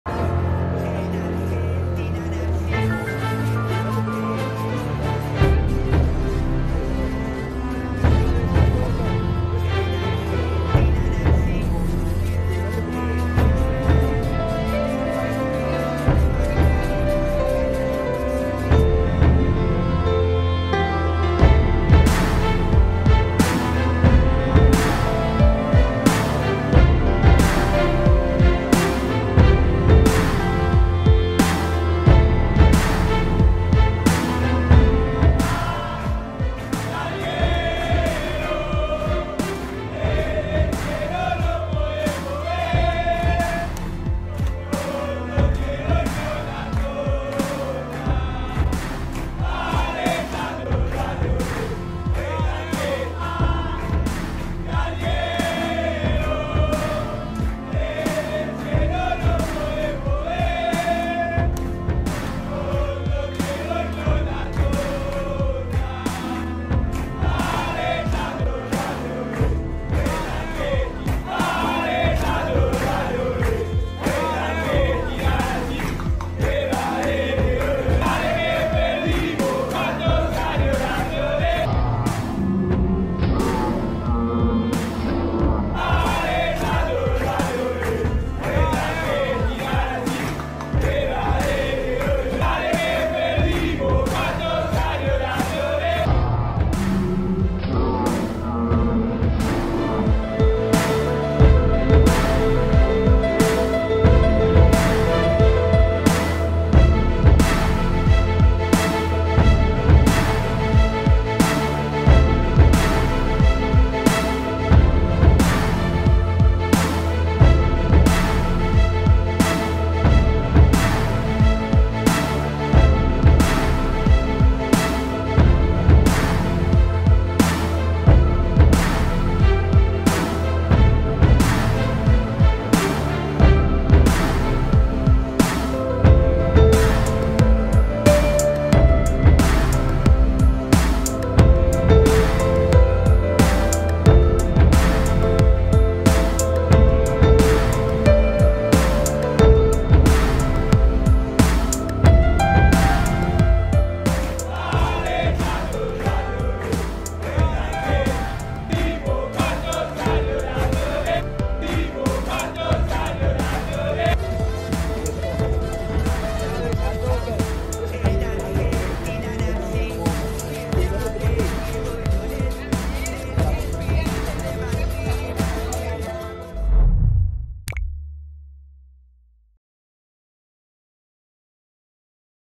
مطاردة زوجة ميسي في مول.. والسبب المكسيك!
حدثت لحظة غير متوقعة لأنتونيلا روكوزا زوجة النجم الأرجنتيني ليونيل ميسي أثناء سيرها بأحد مراكز التسوق في قطر بسبب المكسيك.